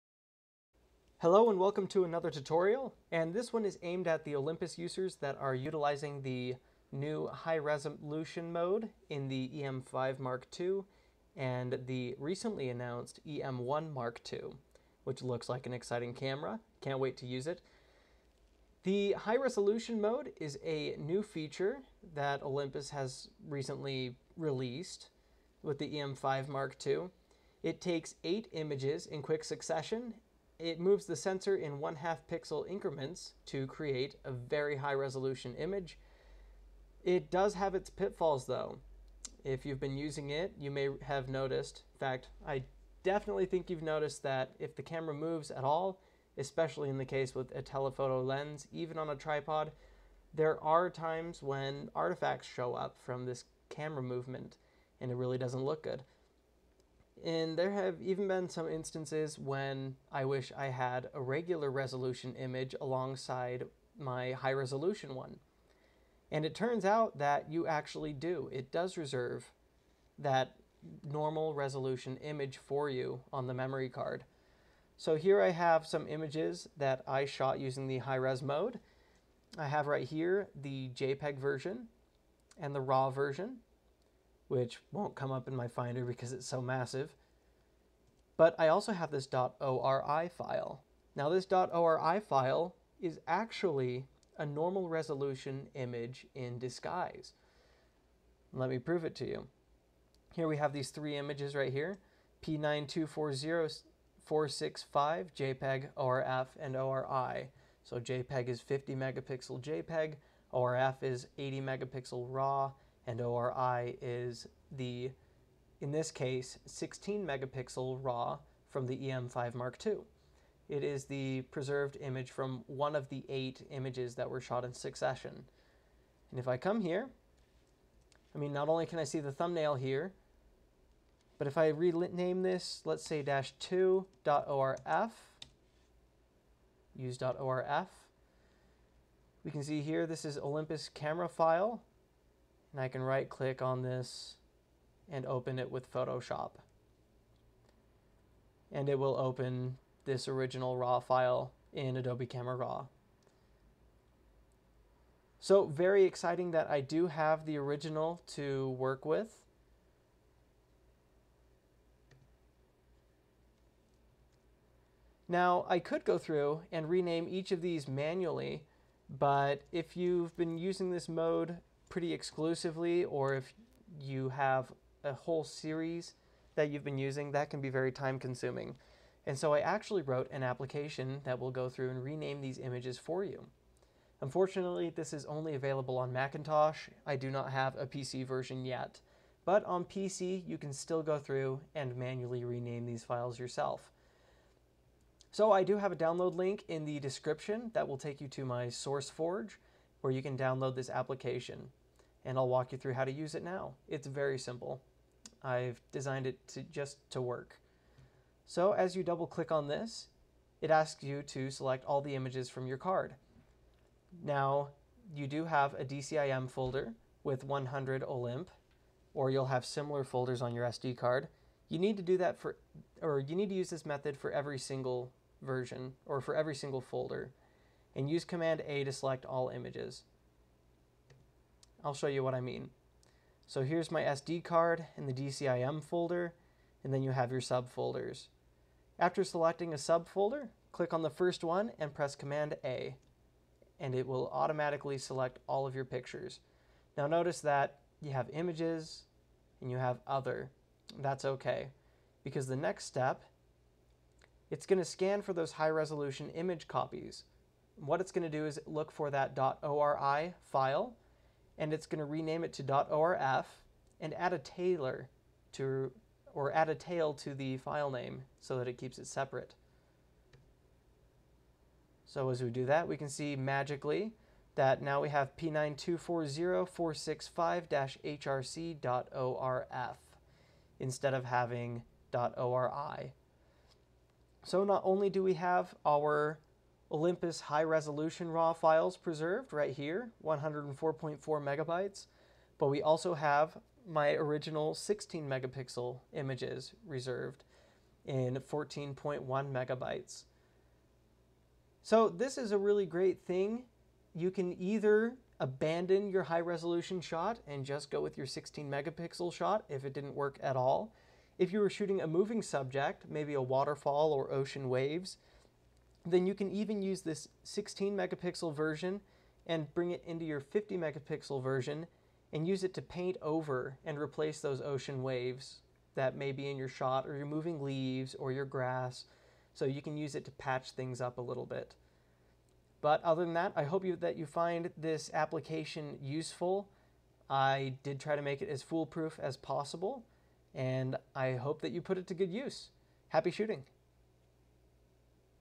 Olympus - Recovering Normal Resolution Files (From High-Res mode)
The Olympus OM-D E-M5 Mark II and (Newly Announced) E-M1 Mark II cameras provide a high-resolution shooting mode that stitches 8 images into one single super high resolution image. This does have its pitfalls. If the camera moves (at all) the image can be completely ruined.

Luckily, Olympus provides a normal resolution raw file in addition to the high-res image! This is disguised as a .ORI file. You can either rename it yourself, or you can use my automatic tool to correct hundreds of files all at once (without affecting the others).

The program can be downloaded from here, and is available for MAC only